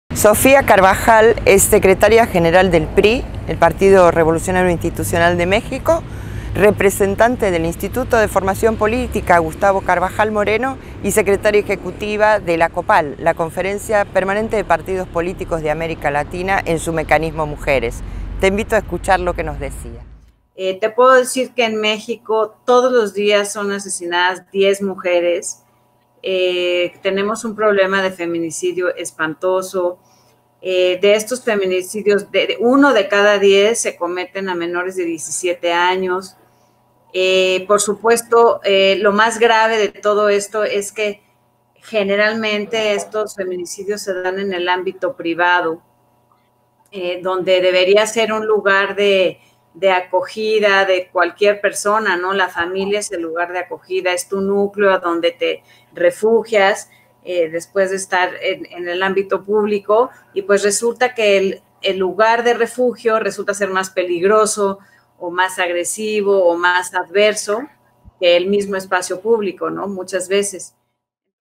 Lo mejor de PalabraIntransigente - Sofia Carvajal
Los feminicidios no son algo que sólo ocurre en nuestro país o en nuestra provincia, sino que es algo lamentablemente universal, y que se magnifica en toda América Latina.

Sofia Carvajal, Secretaria General del Partido Revolucionario Institucional de México (PRI), representante del Instituto de Formación Política "Gustavo Carvajal Moreno", y Secretaria Ejecutiva de la COPPPAL, y esto nos decia respecto de la forma más extrema de la violencia de género en México.